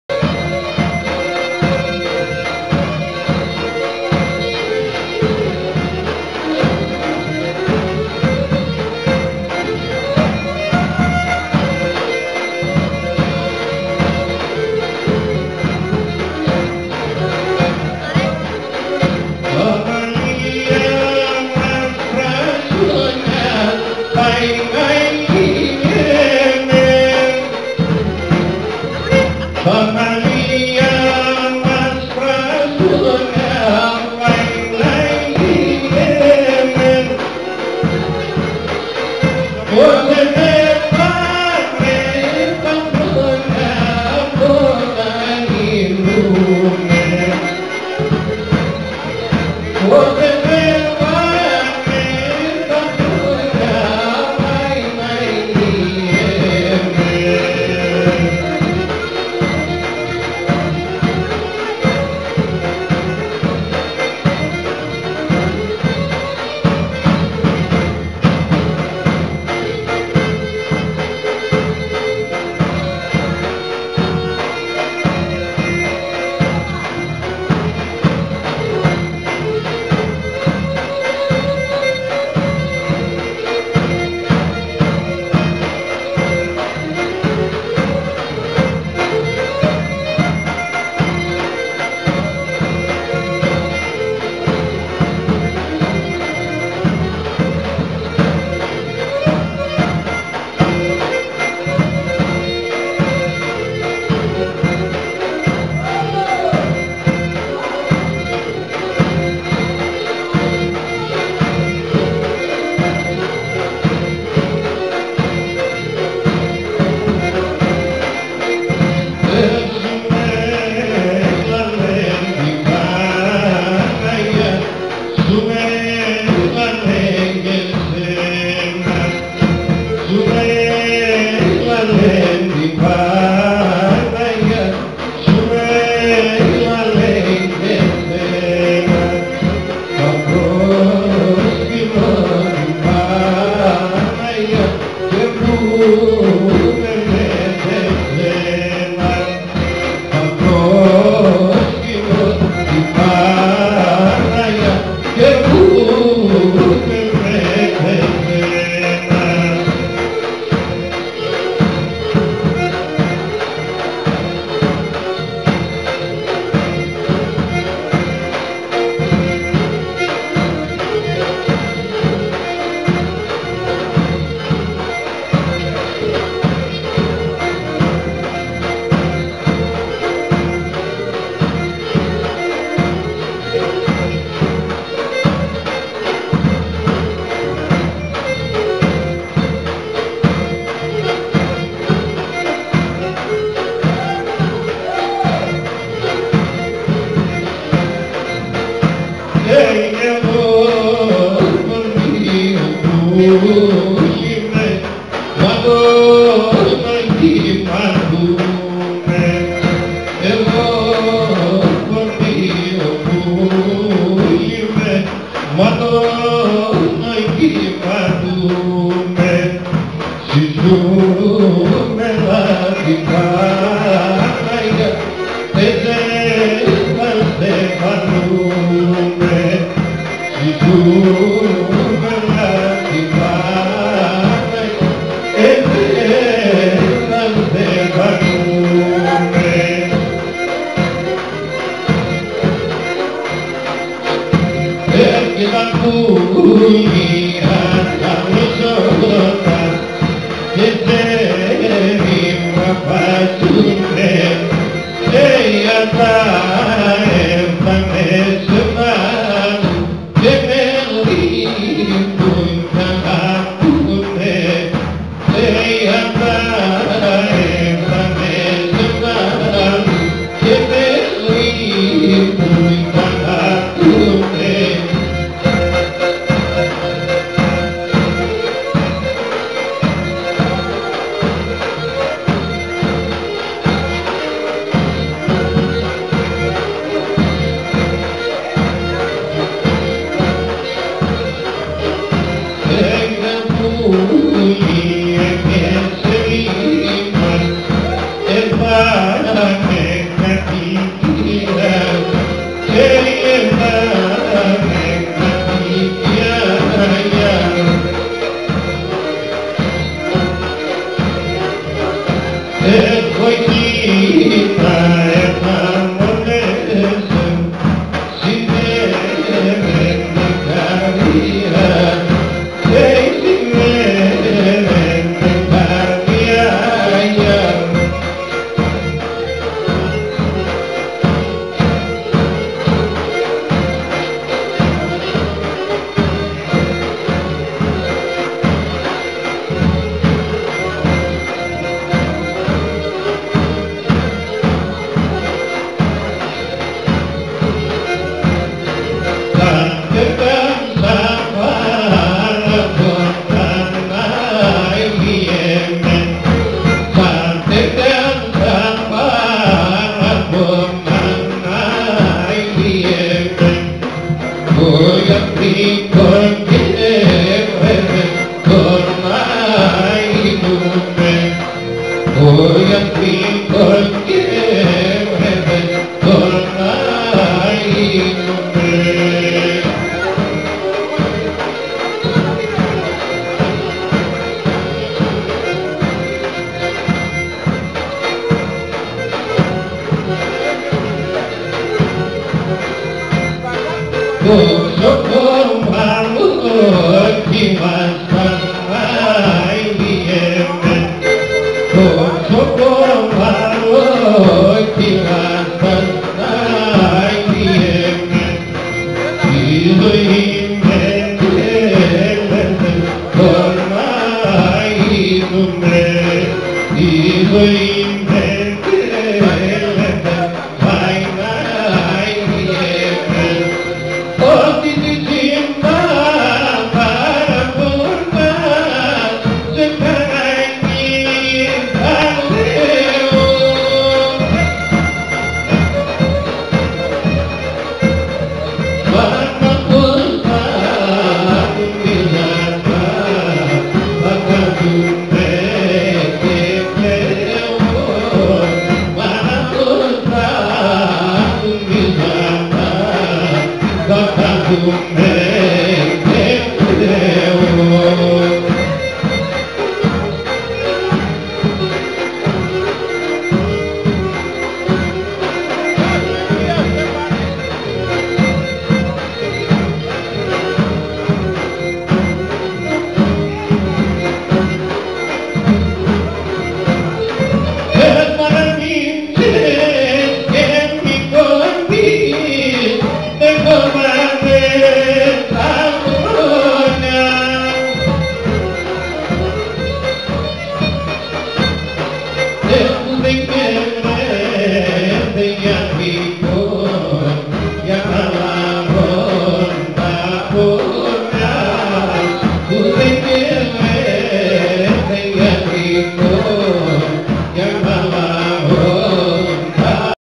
25 12 2013 17 www santasmotos gr
Λύρα: Αμπεσλίδης Γιώργος, Δημήτρης Τσiαβδαρίδης
Τραγούδι:Παριγορίδης Γιάννης
Νταούλι:Παπαδόπουλος Θοδωρής

ΠΟΛΙΤΙΣΤΙΚΟΣ ΣΥΛΛΟΓΟΣ ΠΕΥΚΩΝ «ΟΙ ΡΙΖΕΣ»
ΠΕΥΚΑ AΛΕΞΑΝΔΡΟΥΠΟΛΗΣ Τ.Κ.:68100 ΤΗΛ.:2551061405